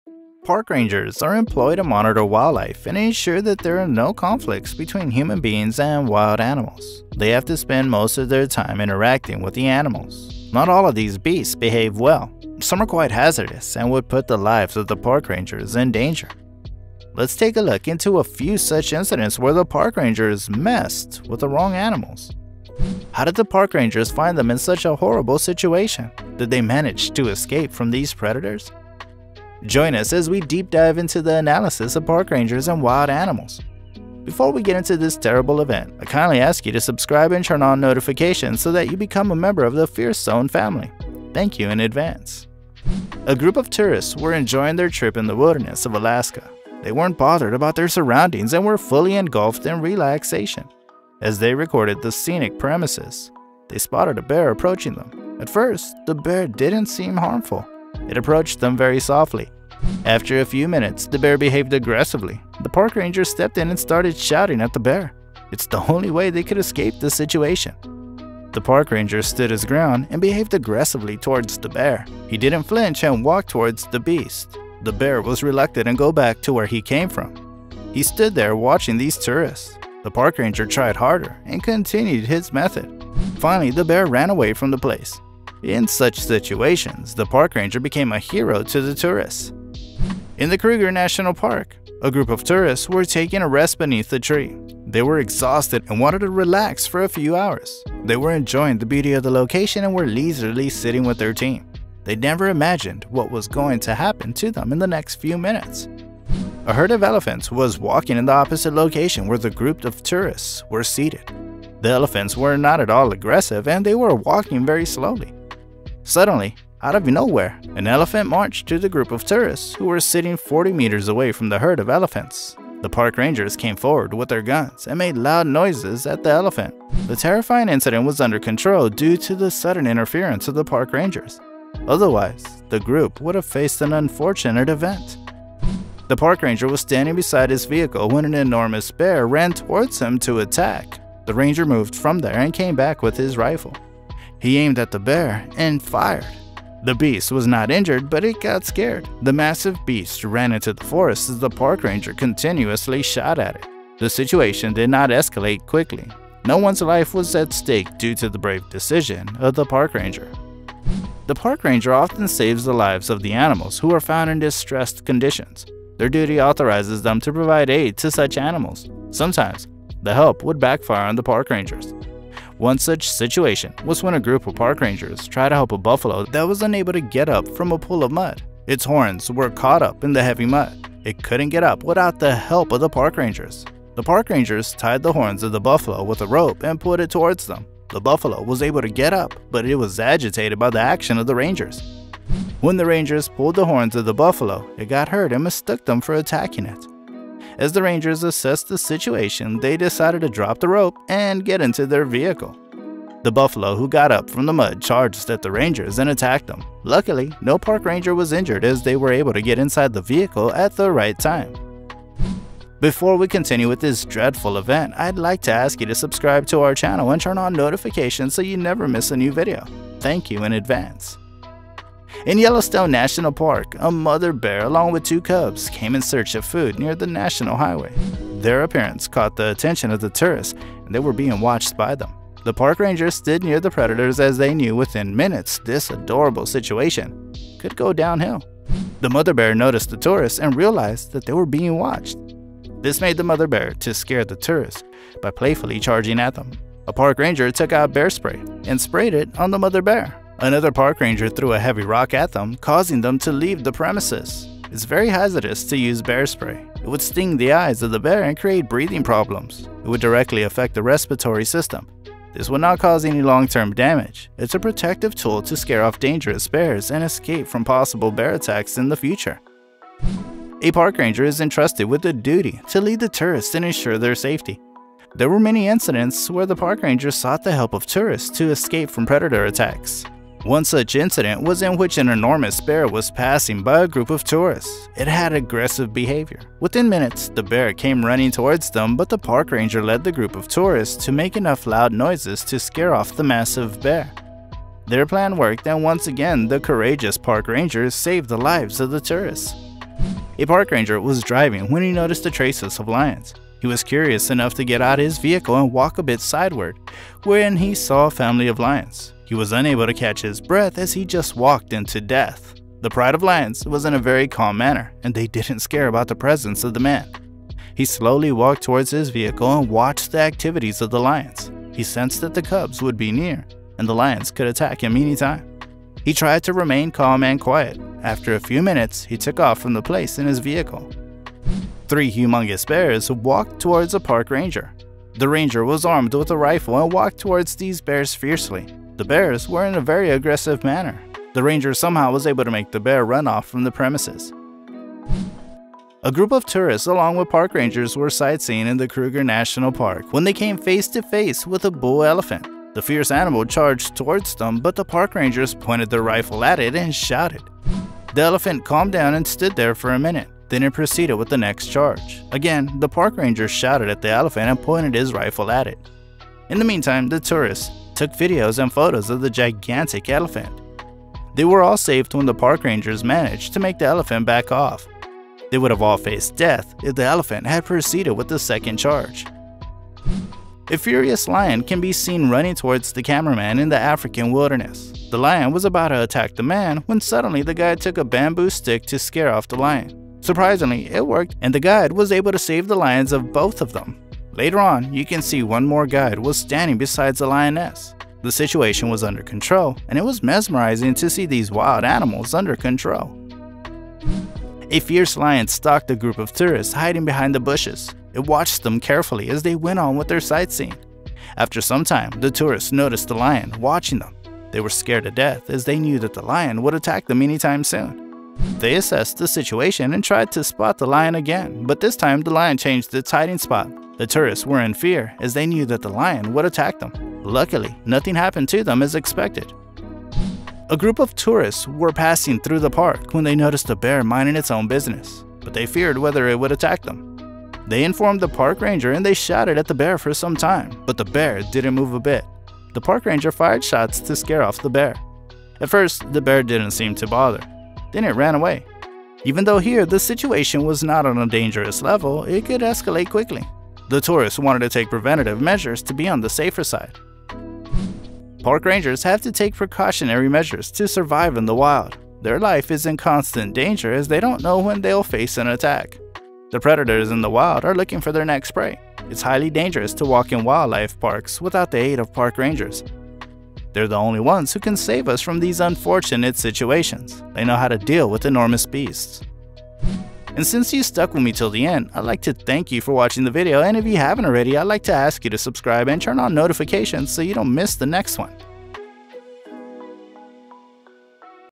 The HORRIFYING Last Minutes of Park Ranger Salifou
WELCOME TO 'FIERCE ZONE':

Park Rangers are employed to monitor wildlife and ensure that there are no conflicts between human beings and wild animals. They have to spend most of their time interacting with the animals. Not all of these beasts behave well. Some are quite hazardous and would put the lives of the park rangers in danger. Let's take a look into a few such incidents where the park rangers messed with the wrong animals.

How did the park rangers find them in such a horrible situation? Did they manage to escape from these predators?

Join us as we dive deep into this analysis of park rangers and wild animals.